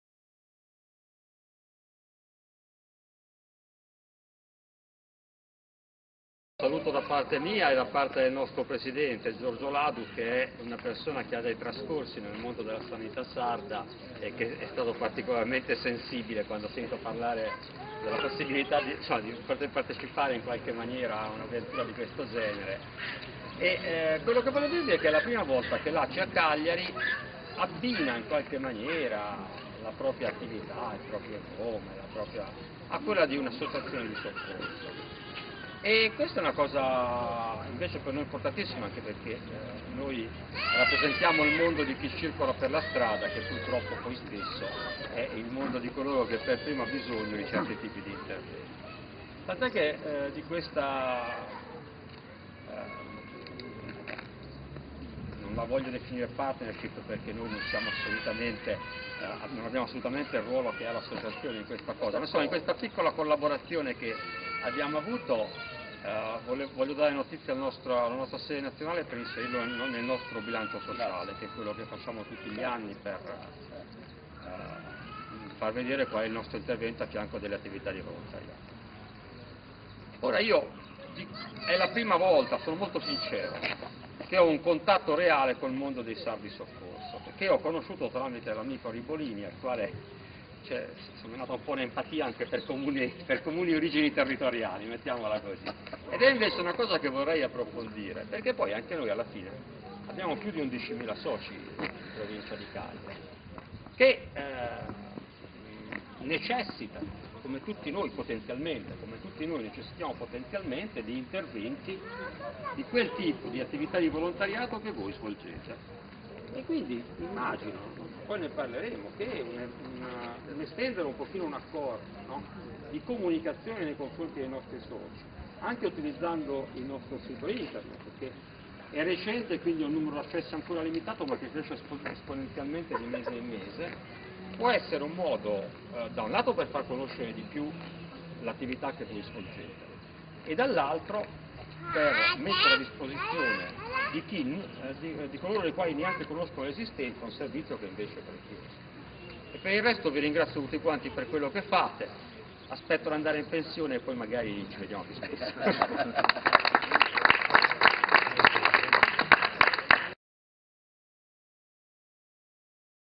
Cagliari: i saluti dell'ACI a "I Sardi Soccorso"
I saluti del Direttore Generale dell'Aci, Alessandro Paita a "I Sardi Soccorso" durante la presentazione della nuova ambulanza nella sede dell'associazione di volontariato in via Mandrolisai.